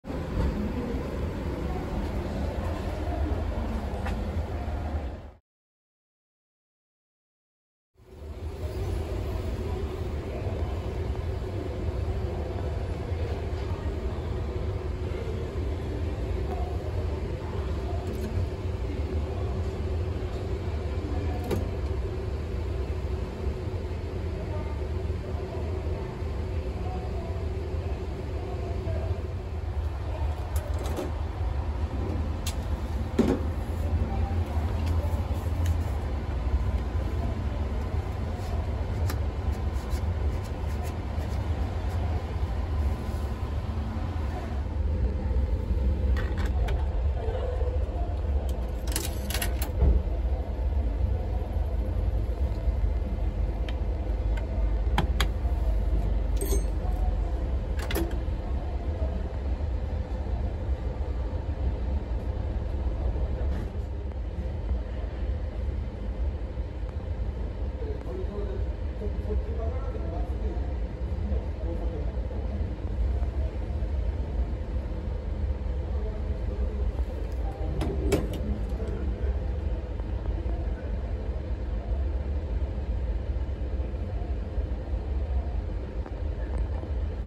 DMC-8A公衆電話機 カード不可の画面表示 倉敷駅南口
0:00 外観
0:39 受話器を上げてもカードは投入できませんでした
0:51 受話器を上げて硬貨を投入
0:59 発信し10円1枚が収納される
1:04 残り時間が短くなると点滅開始
1:11 残り時間がなくなり点滅が止まる